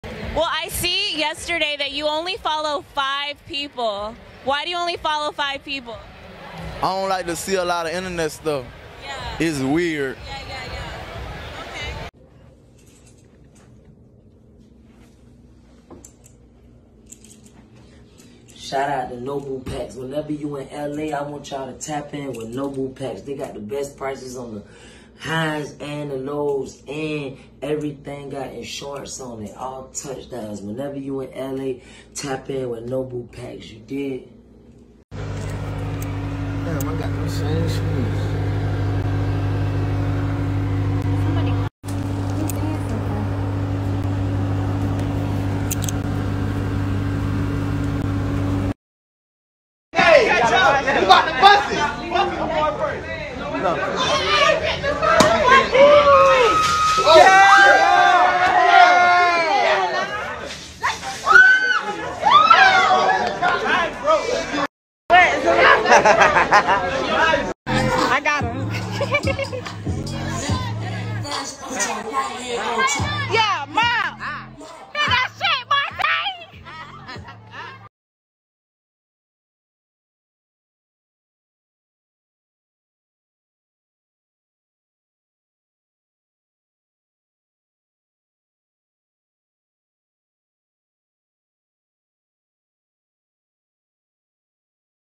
Quando Rondo Pulls Up To Lul Pab Funeral All The NH Crips Pop Out
Quando Rondo Lul Pab Funeral